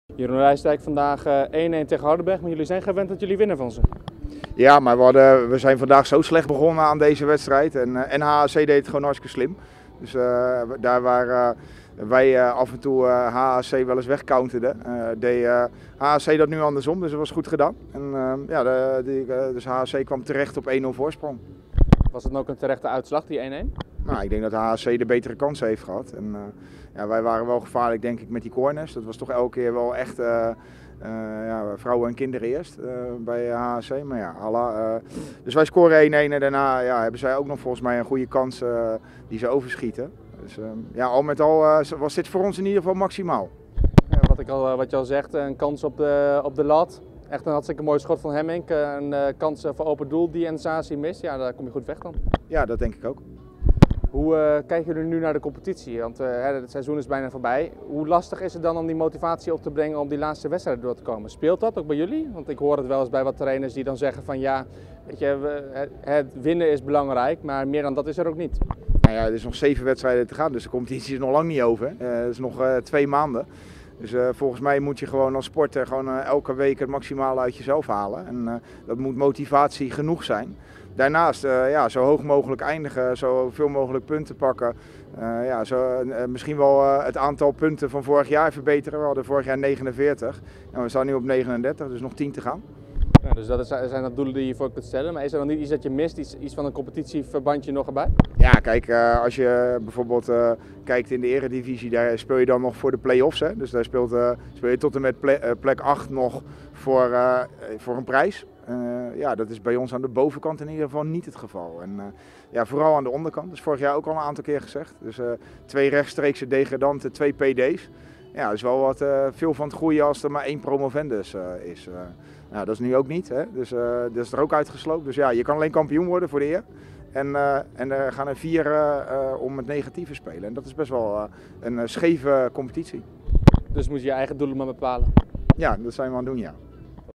Rijsdijk: 'Nog tien punten te gaan'
Jeroen Rijsdijk wil dit seizoen met Excelsior Maassluis het puntenaantal van vorig seizoen minimaal proberen te evenaren.